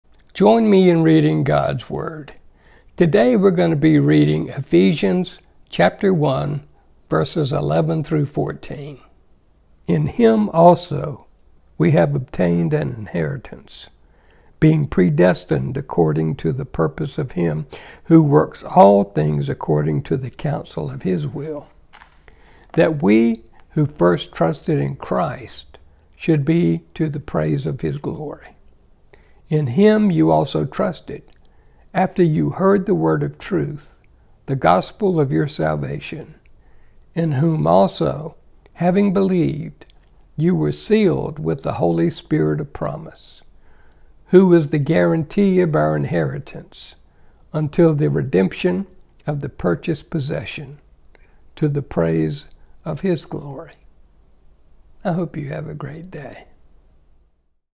READING GOD'S WORD - EPHESIANS 1:11-14 - YOU ARE SEALED BY THE HOLY SPIRIT
When you trust in Jesus Christ as your Savior, you are sealed by the Holy Spirit as a guarantee of your salvation.